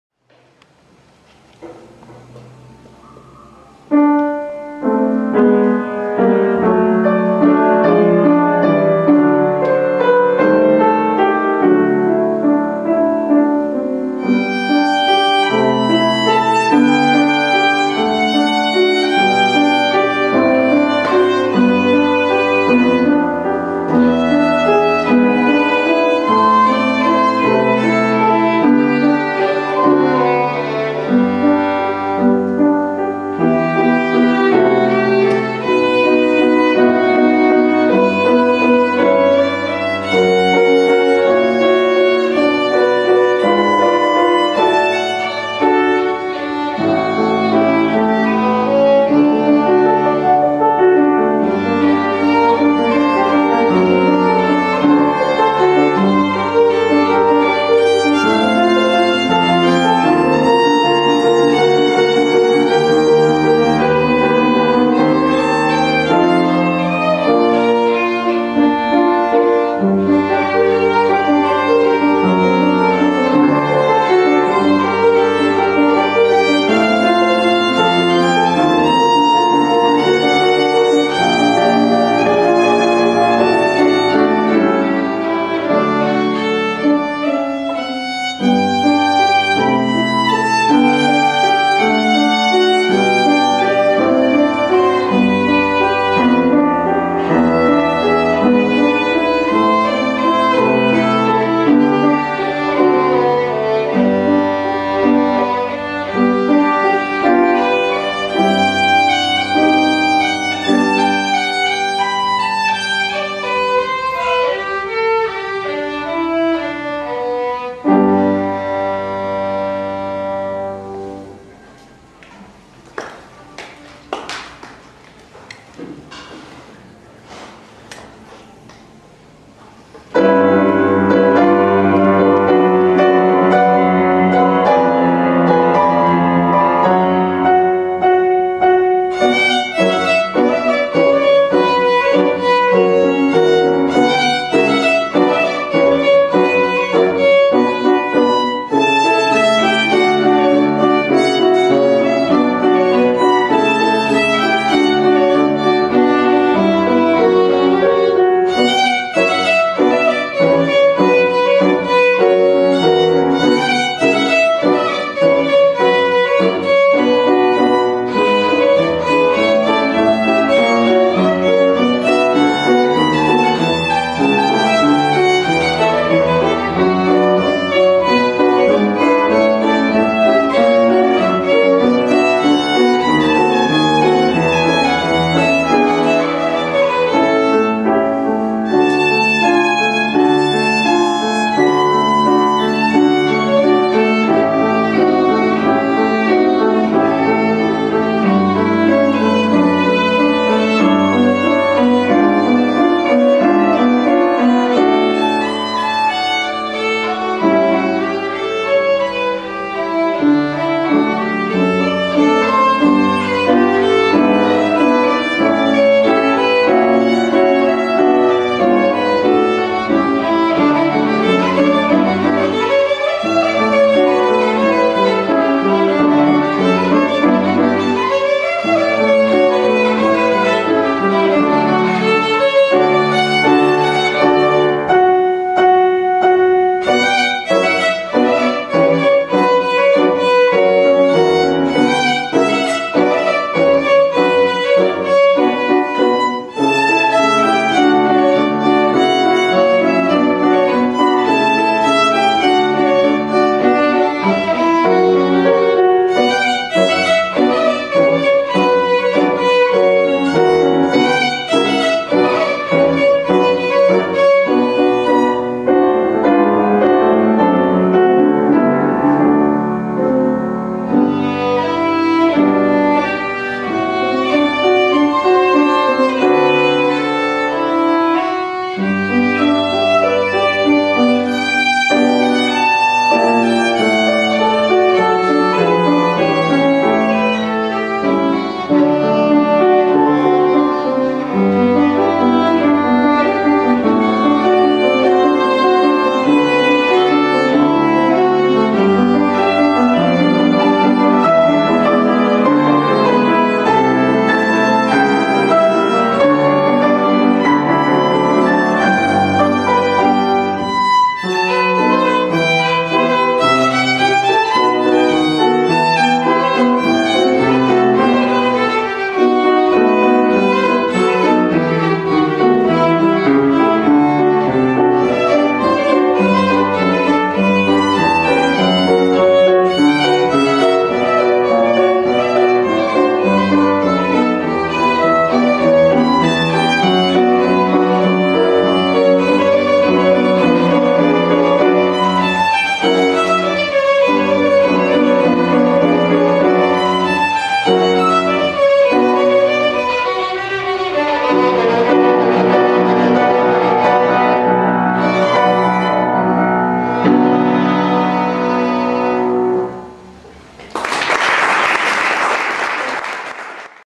Panka. 2011.04.19.
Hegedű tanszaki koncert a II. ker. zeneiskolában